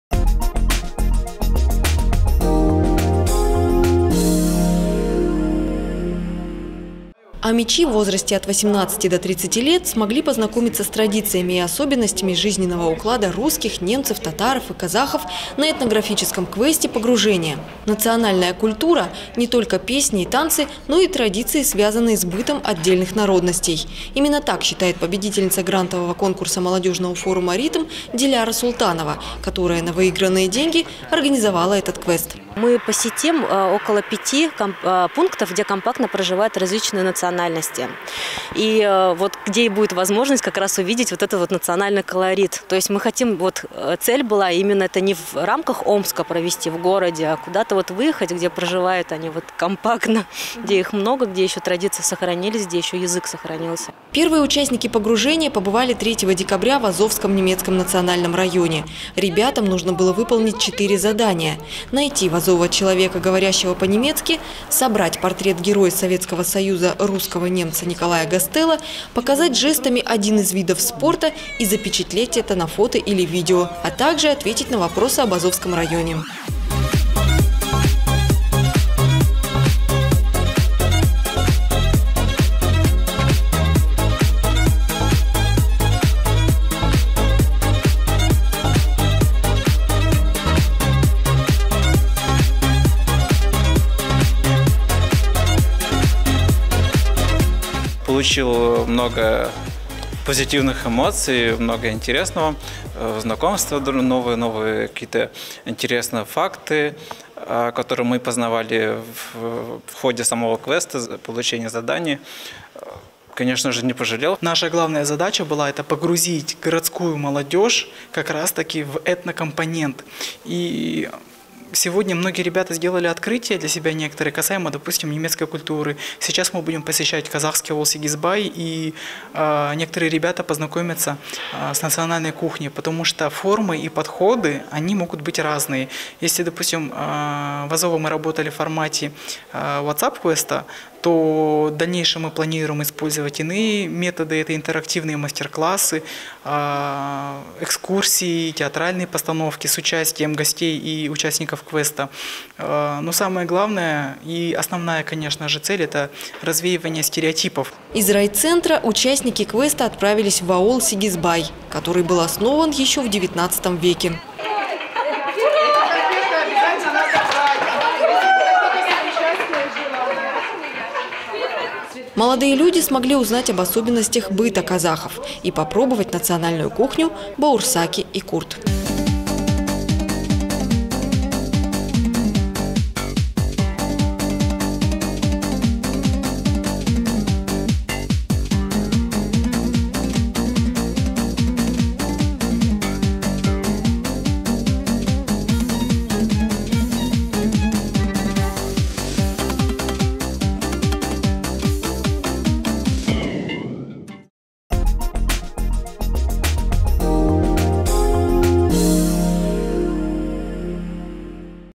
Проекты молодежного форума «РИТМ»: этнографический квест «Погружение»
Национальная культура – не только песни и танцы, но и традиции, связанные с бытом отдельных народностей. Именно так считает победительница грантового конкурса молодежного форума «РИТМ» Диляра Султанова, которая на выигранные деньги организовала этнографический квест «Погружение»